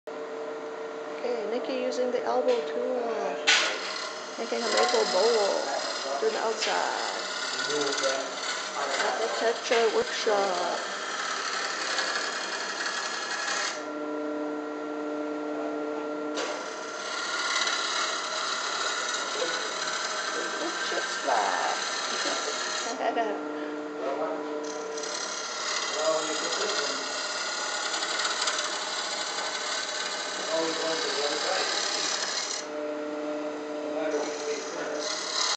Nicki using Elbo Tool on outside of bowl July 2 2019
client at the Tetra Wood Working shop using the Elbo Tool, which was generously donated by master woodturner and inventor Tim Yoder